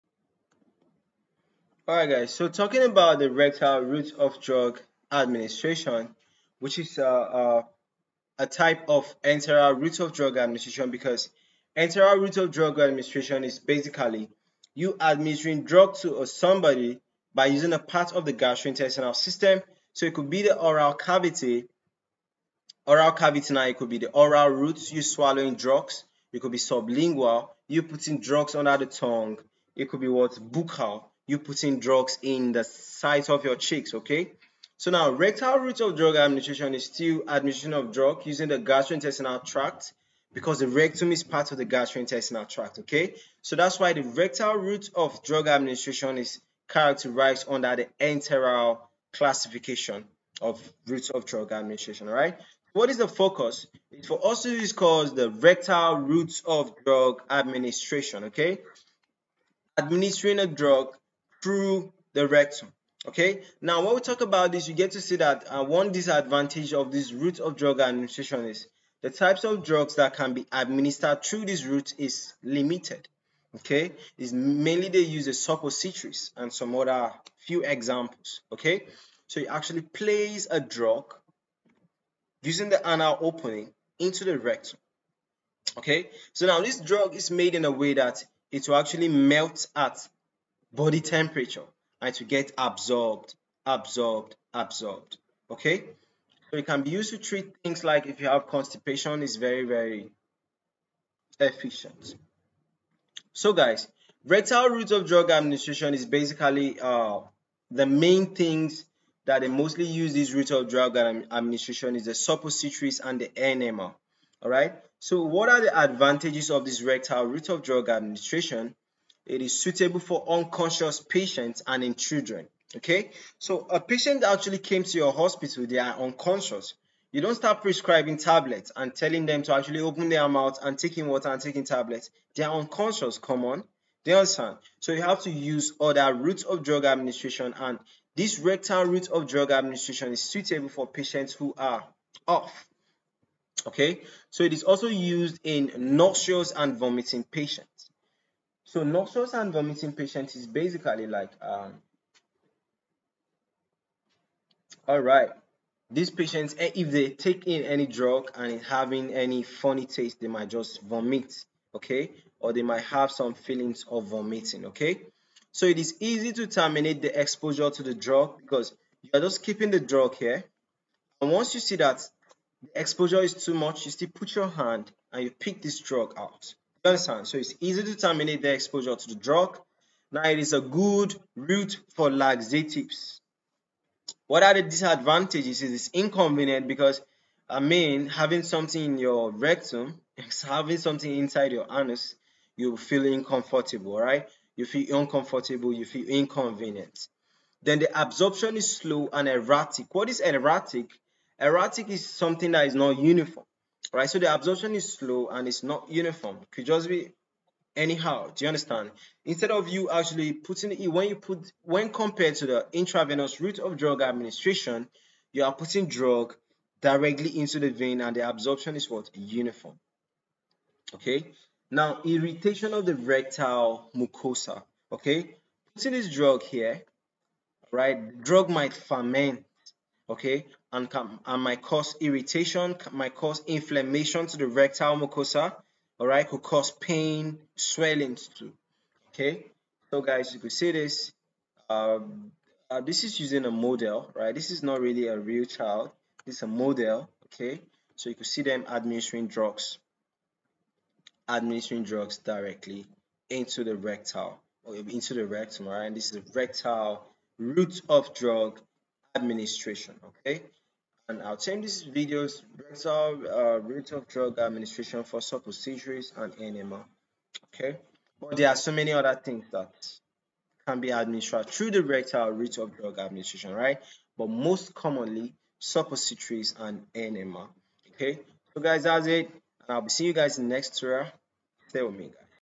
Rectal Route of drug administration for Suppositories and Enema
Rectal route of drug administration - suppositories or enema

Advantages of rectal route

• Suitable for unconscious patients and in children

• Used in nauseous and vomiting patients
• It is easy to terminate exposure to the drug

• It is a good route for laxatives

Disadvantages of rectal route
• Inconvenient

• Absorption is slow and erratic

• Irritation to rectal mucosa